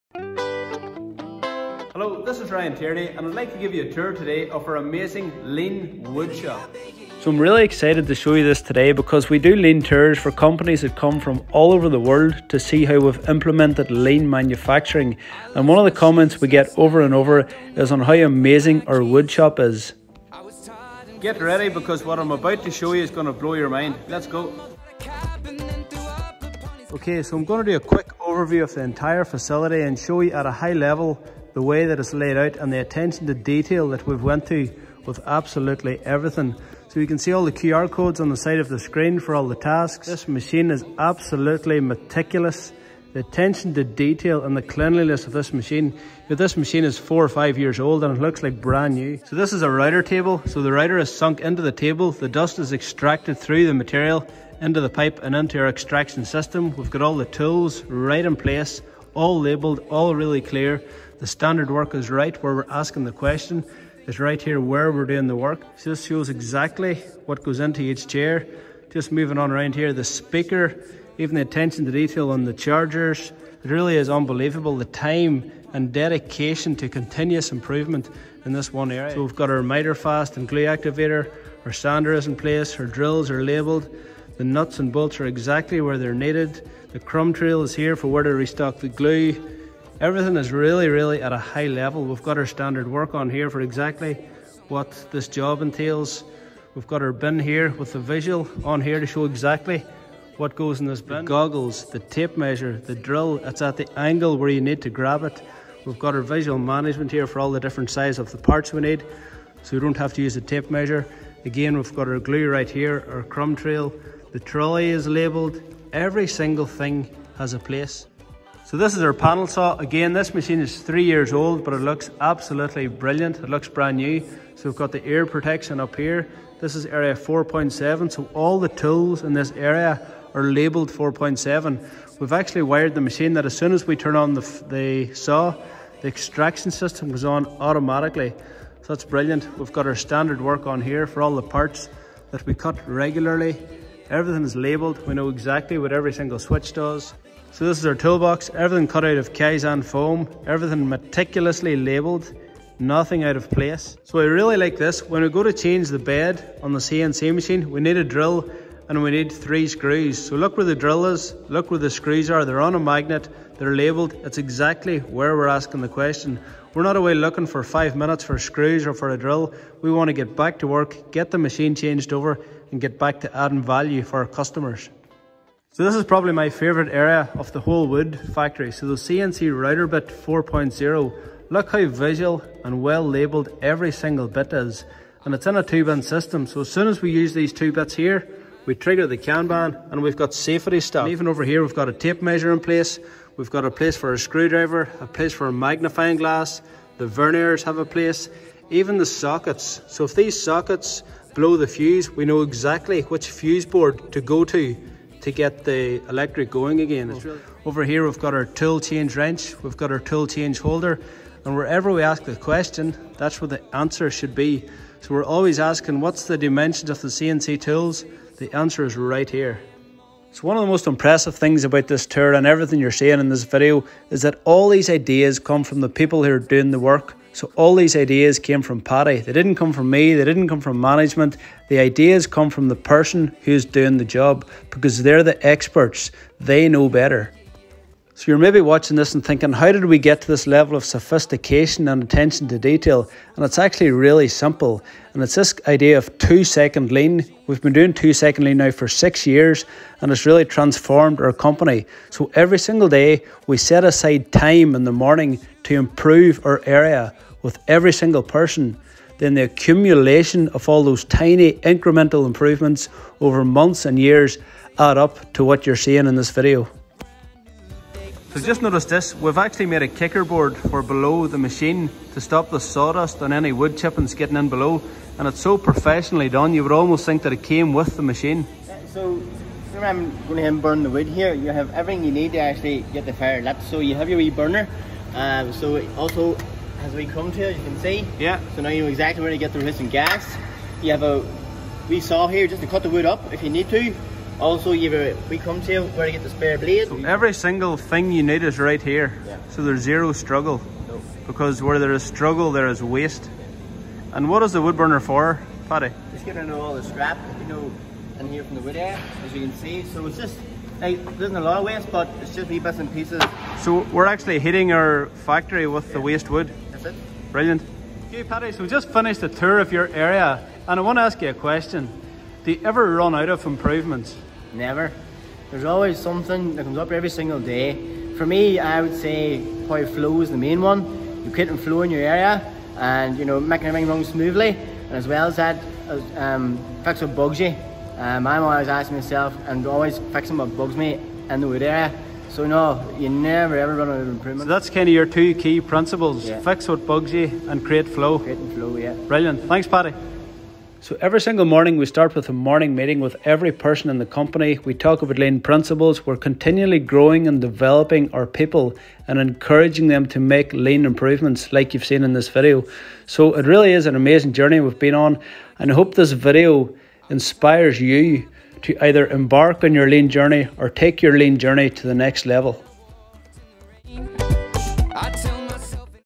Amazing Lean Woodshop Tour
Want to see a world class lean woodshop? My goal for this video is to inspire you to embark on your lean journey…or to take your lean journey to the next level…the results will amaze you!